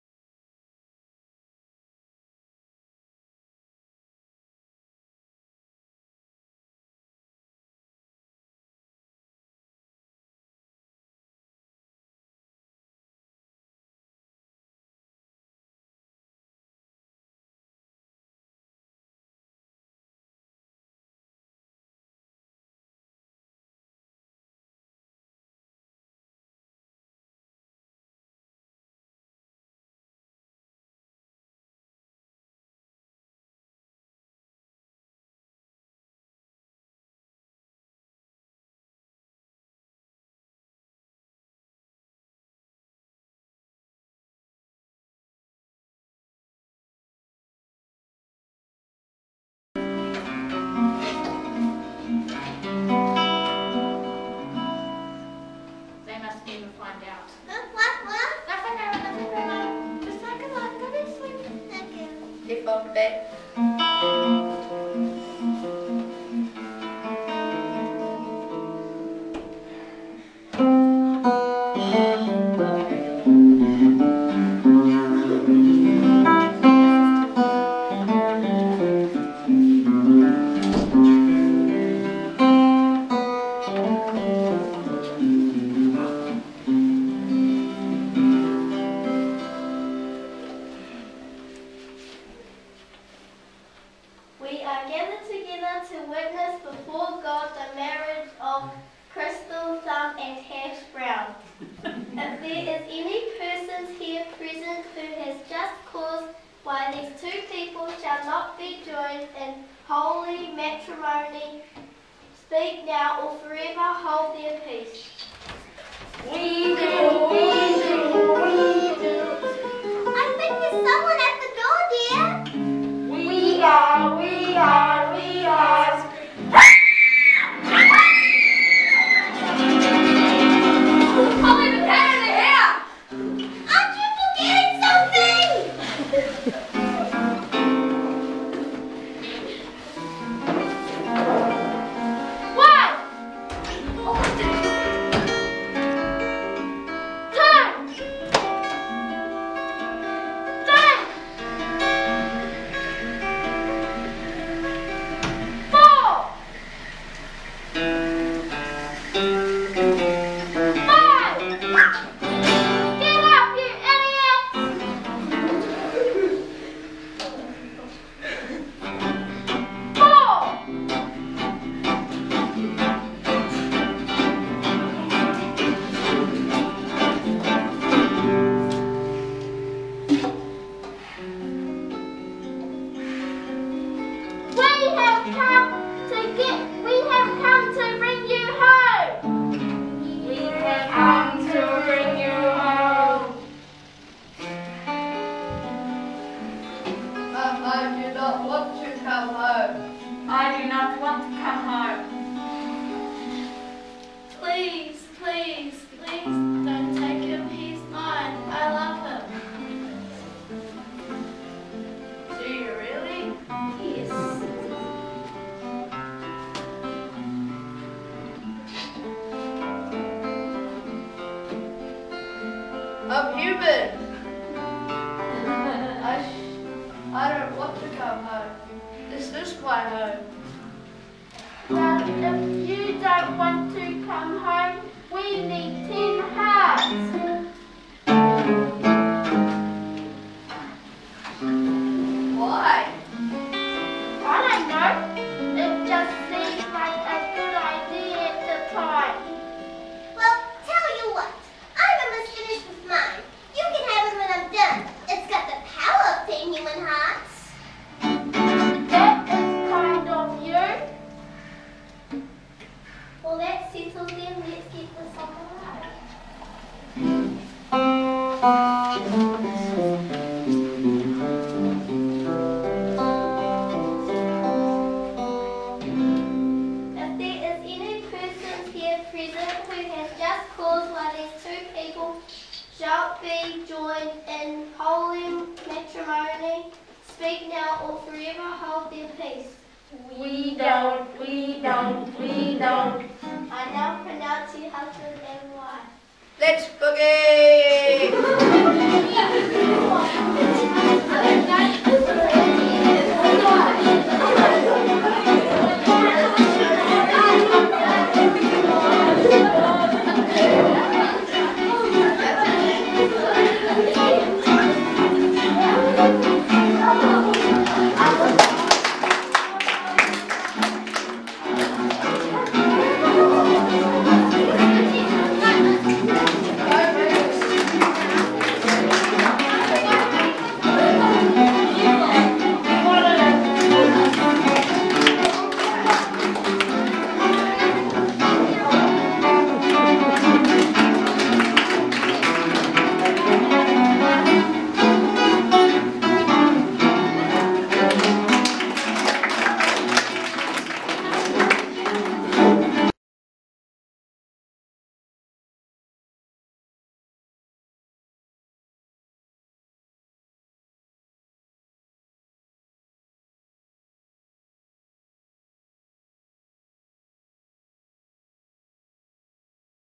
Tairawhiti R.E.A.P
Gifted and talented programme for yrs 6-8. Improv theatre